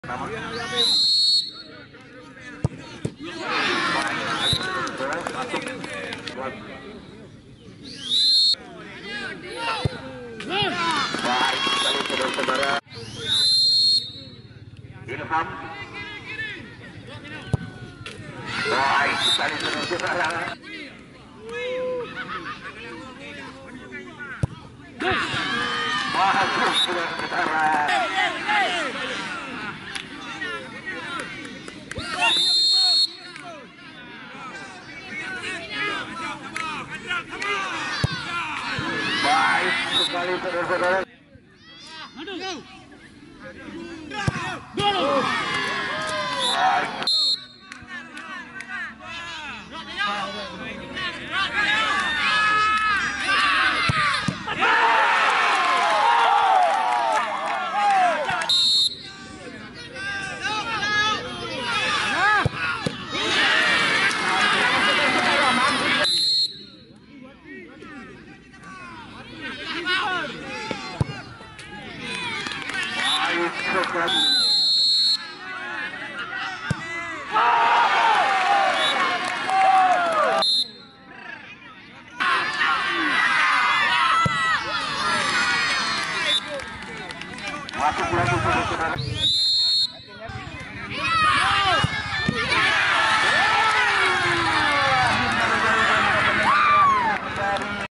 DRAMA ADU PENALTI | AY FC VS RAP MANOK
DRAMA ADU PENALTI TARKAM PEMAIN BINTANG | AY FC VS RAP MANOK.
PIALA PEMUDA CUP 2 ALUE NIBONG, PEUREULAK, ACEH TIMUR